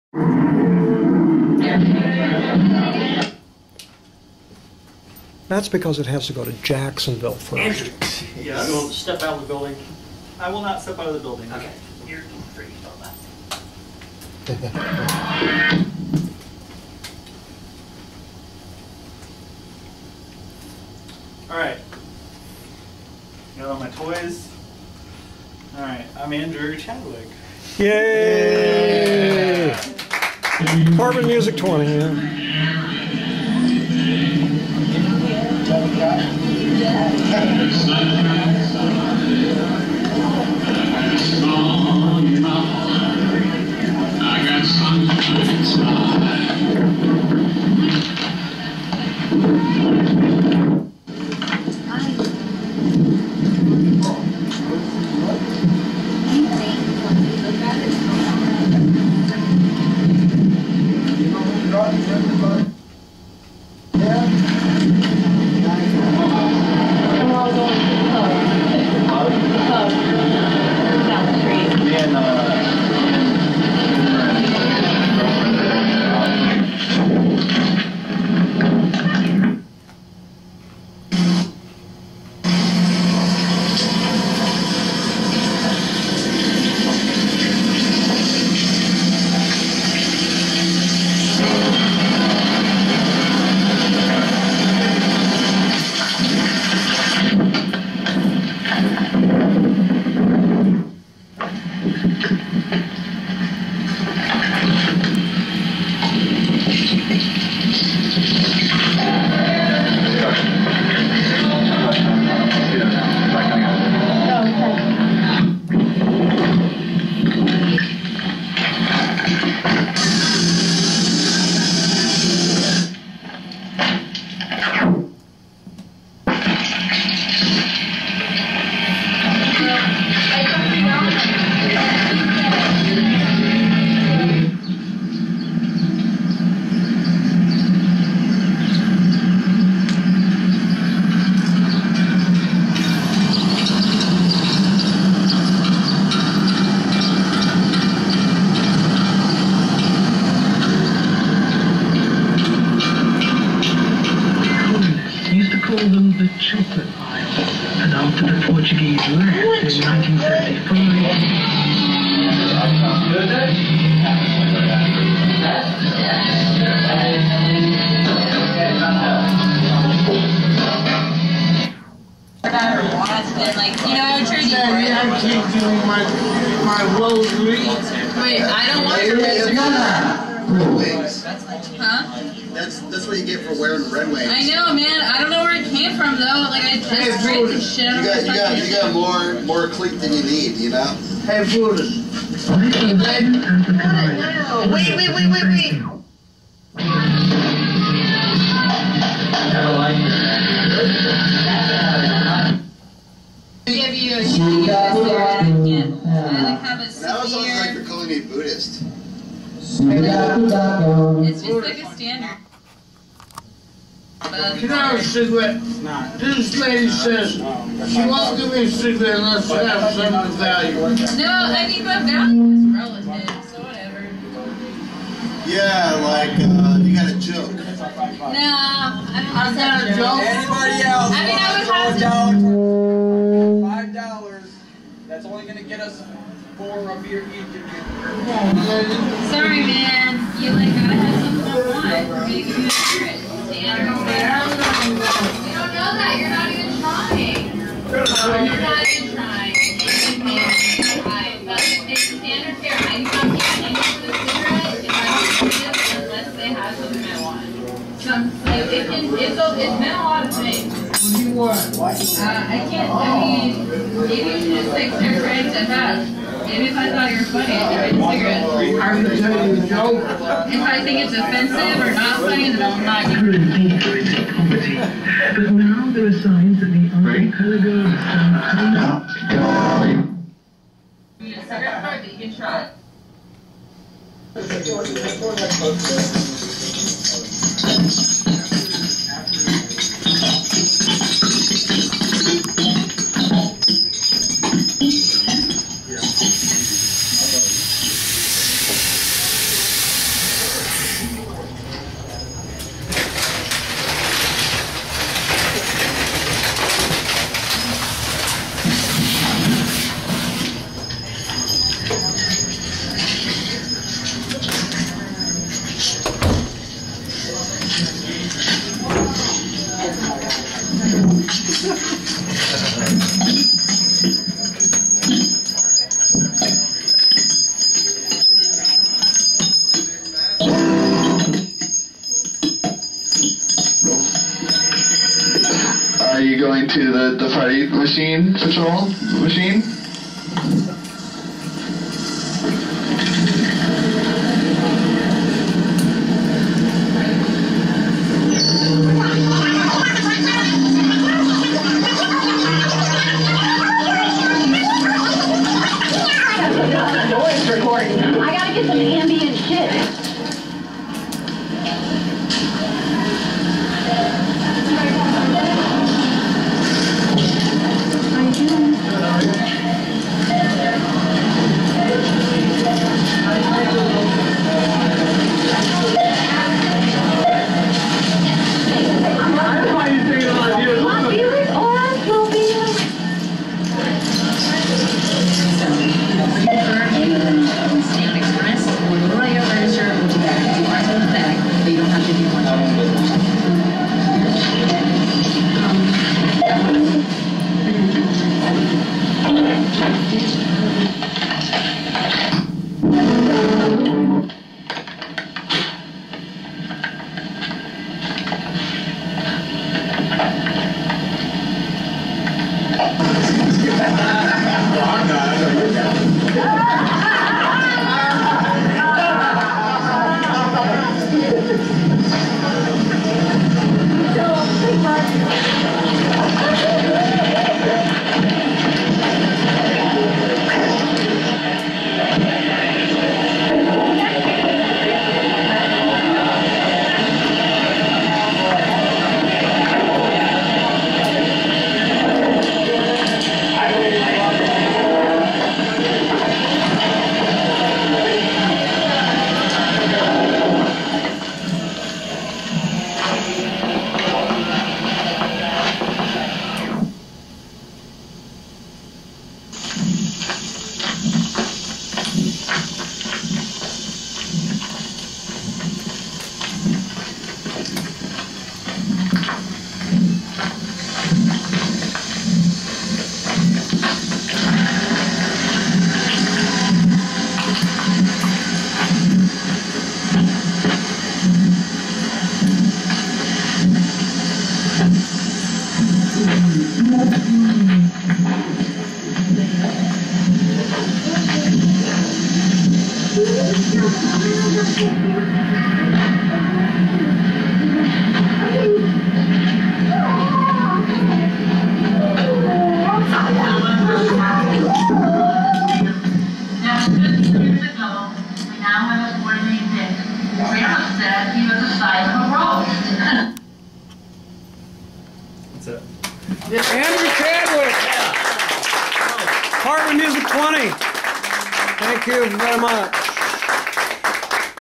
Andrew Chadwick at Apartment Music #20 improvisation
Andrew Chadwick at Apartment Music #20, Saturday, October 12, 2013, Hal and Mark McGee's apartment, Gainesville, Florida, USA. Apartment Music is a long-running series of shows curated by Hal McGee featuring improvisation, experimental and noise music. This was the fifth performance at AM20.